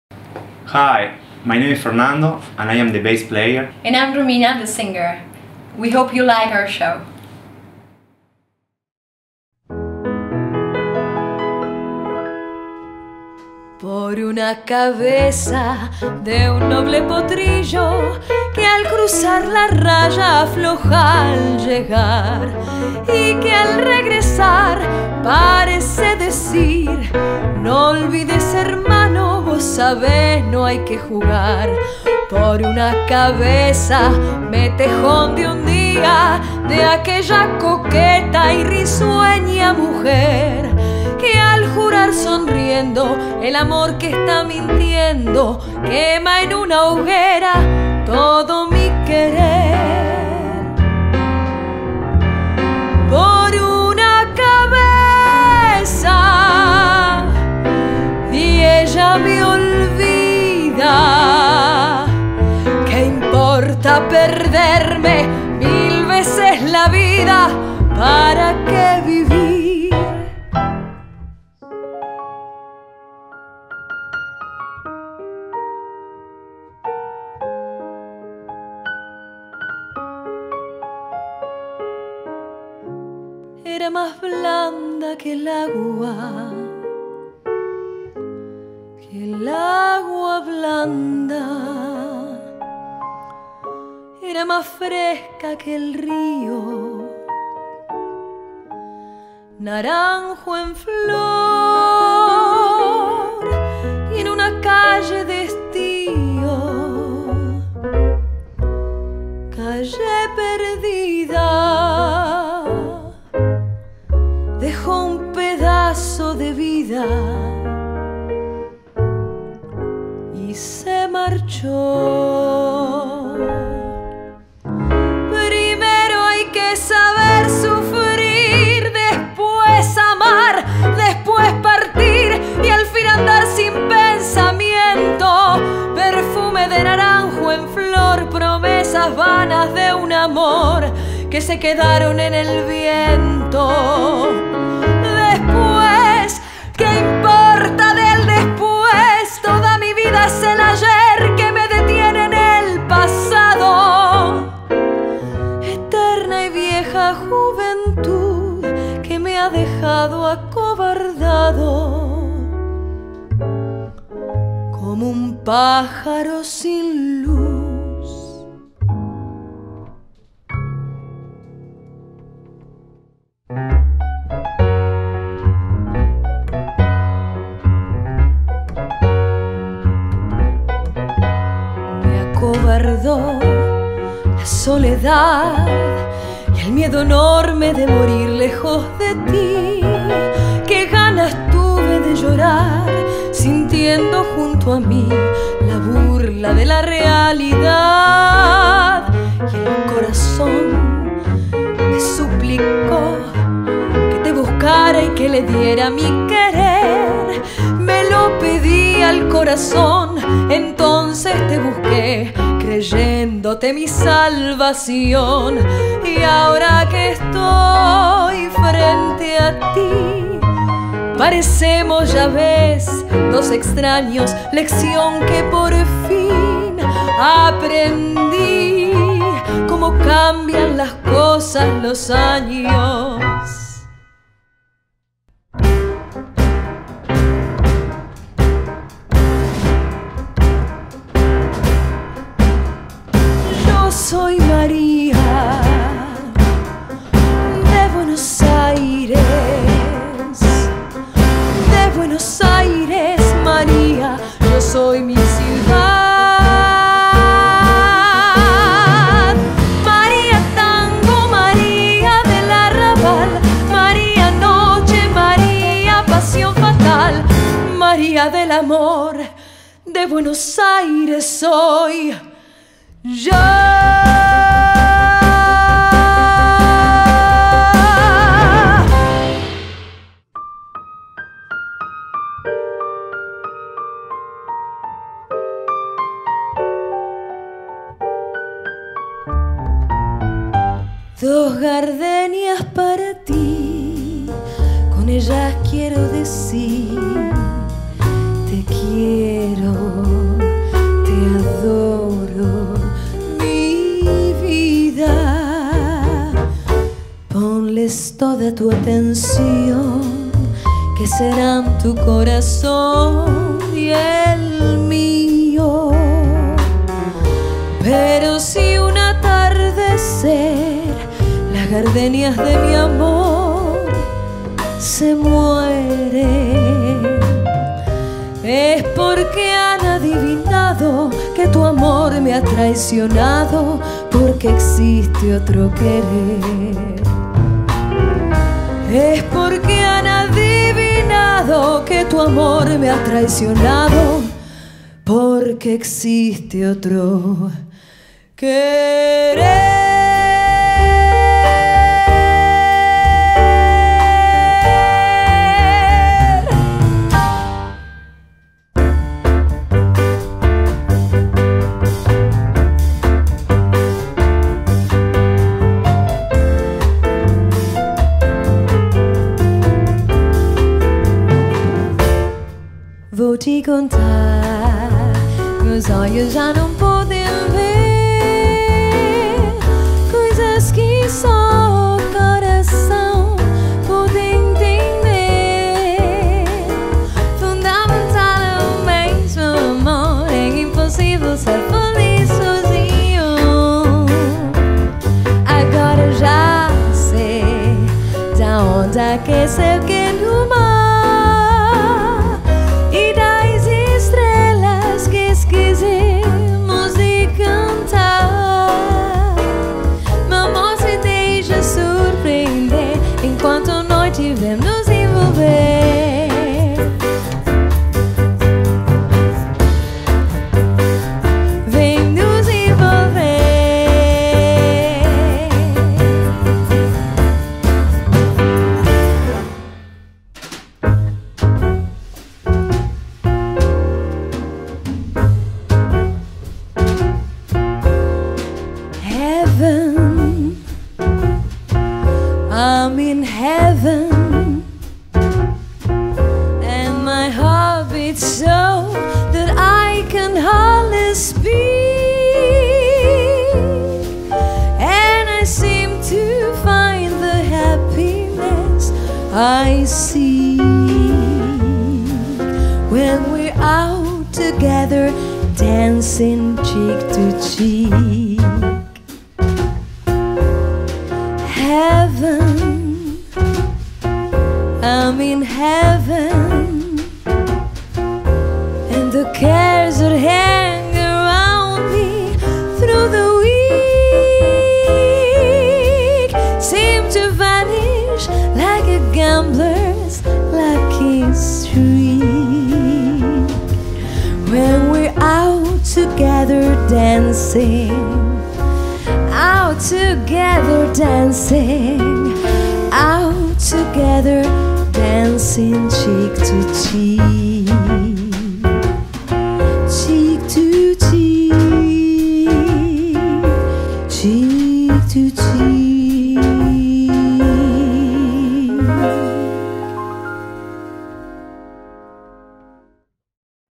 MALBEC - Tango / Bolero / Bossa Nova /Jazz
What makes MALBEC different from any other musical experience is its excellence and versatility.

You will be able to choose which style suits the celebration best!
A wide variety of musical genres will be available for you.

From Jazz, Bossa, Bolero, to Tango and South American music setting the mood for a cool, unique reception.